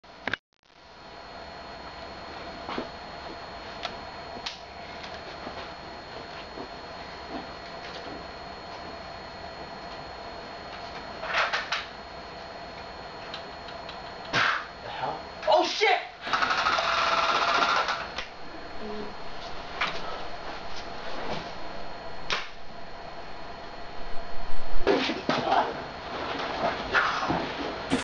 The Real Life Timeshift ae
Based on the game timeshift. sorry about the corney blood.